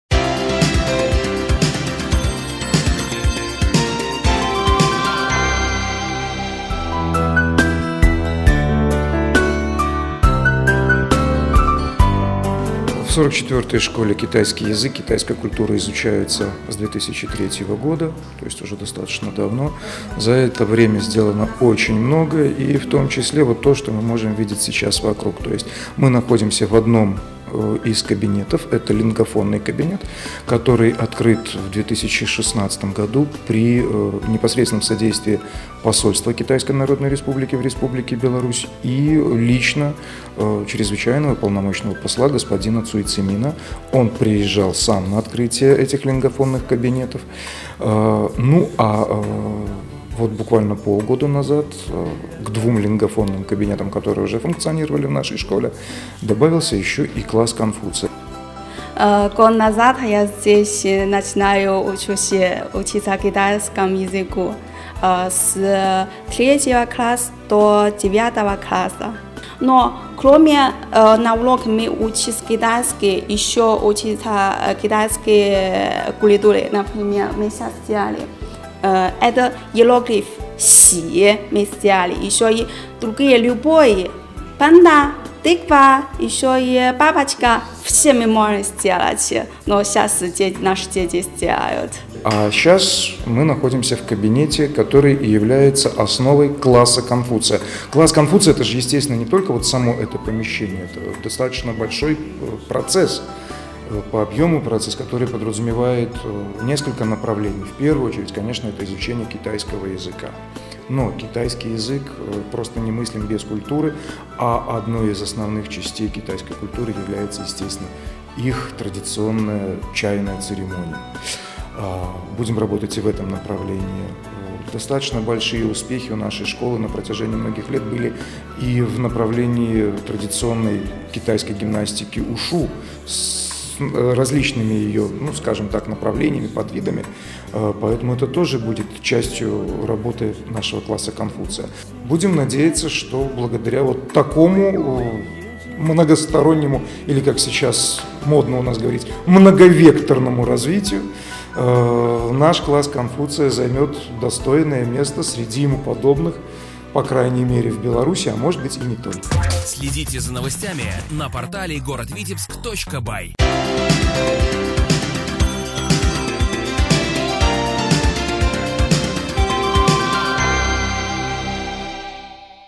Класс Конфуция приглашает в СШ №44 города Витебска.
Класс Конфуция открылся в 44-ой витебской школе, которая одной из первых в Беларуси ввела в программу обучение китайскому языку. Сначала занятия велись факультативно, а с 2007-го язык Поднебесной в профильных классах изучают как основной иностранный. Класс Конфуция создан при поддержке посольства КНР в Беларуси и Пекинского института китайского языка.